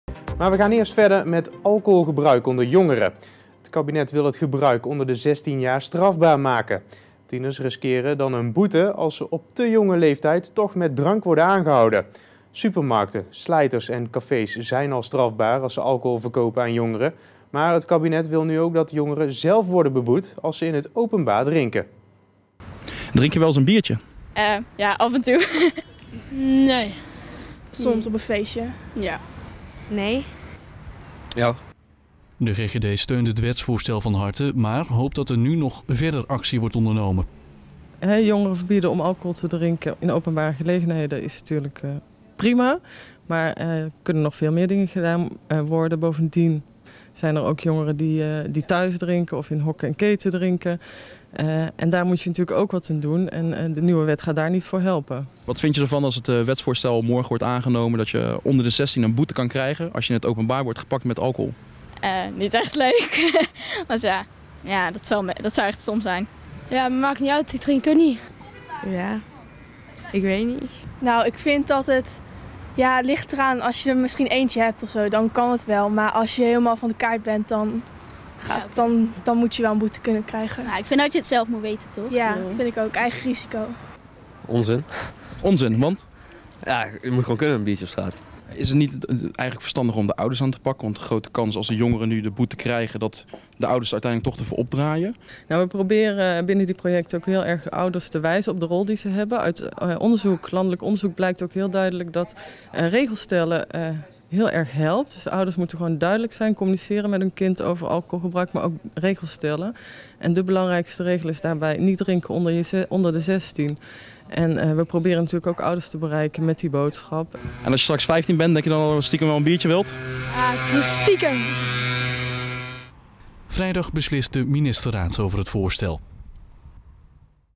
Nieuw alcoholbeleid voor jongeren onder de zestien
Het kabinet wil alcoholgebruik onder de zestien jaar strafbaar maken. Tieners riskeren dan een boete als ze op te jonge leeftijd toch met drank aan worden gehouden.